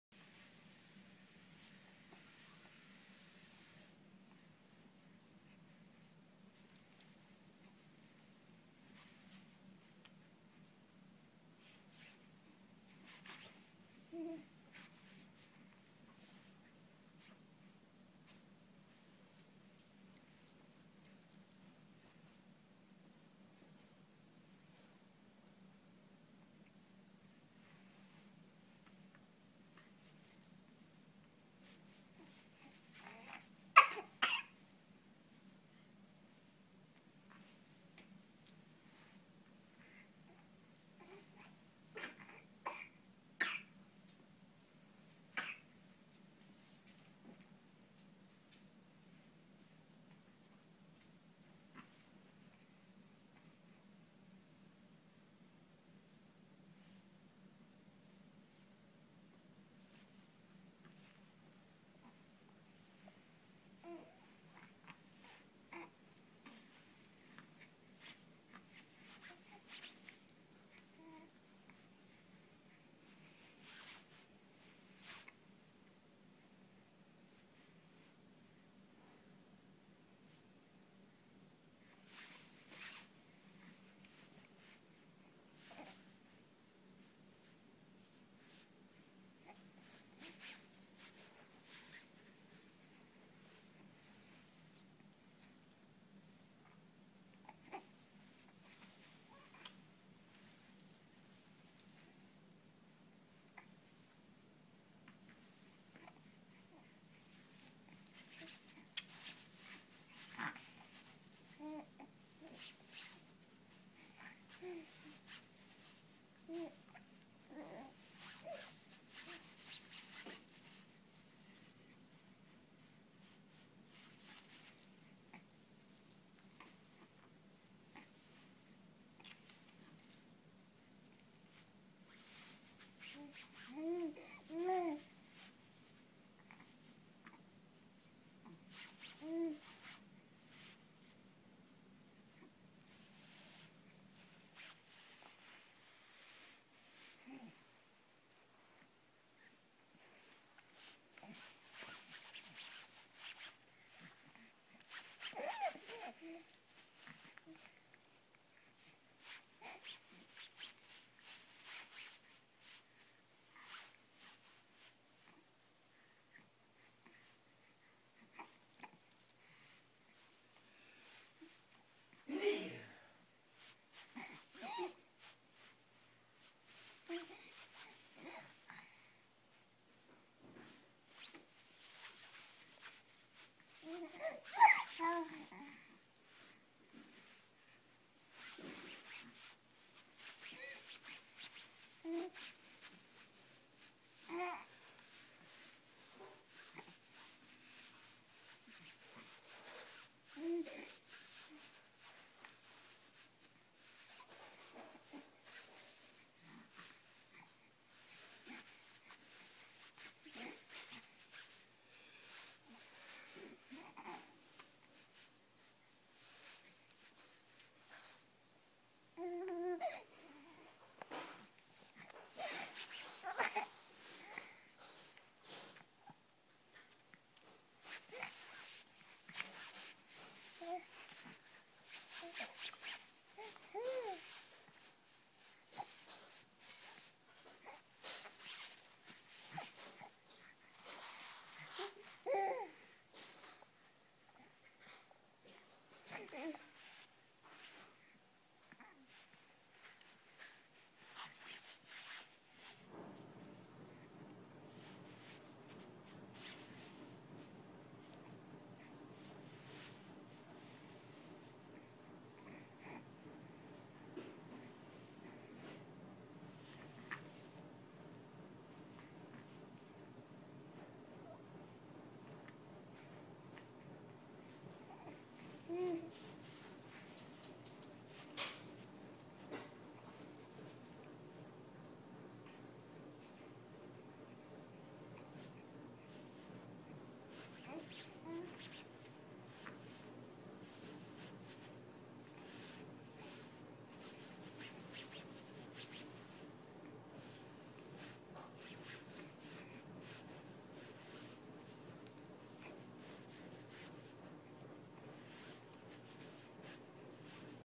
Almost can roll over
Emily is learning to pull up her legs and kick and can almost roll over